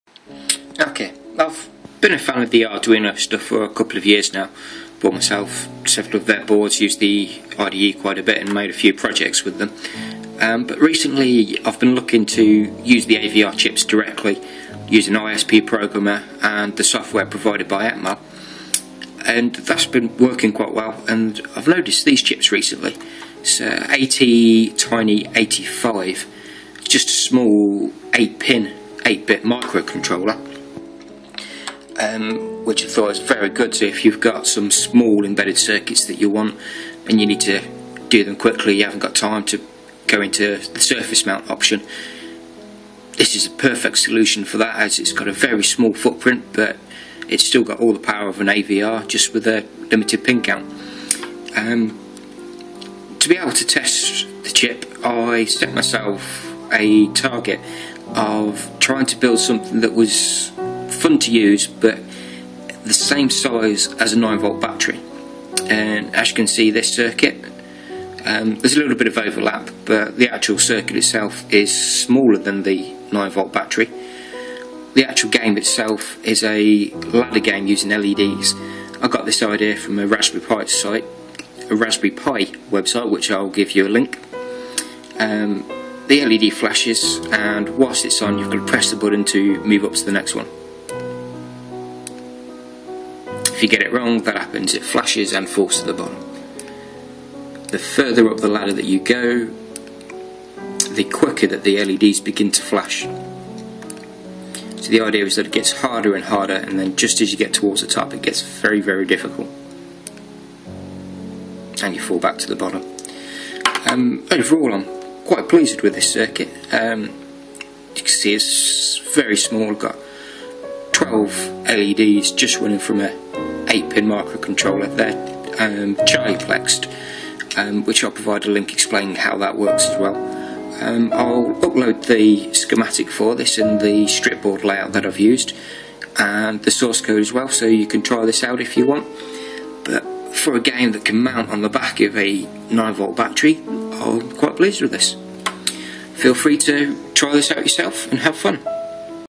LED ladder game running on a ATtiny85
A very small yet addictive game running on an ATtiny85 that is mounted onto the back of a 9v battery.

but I can't grantee I have made any mistakes so the usual disclaimer applies, this is a guide, follow it at your own risk.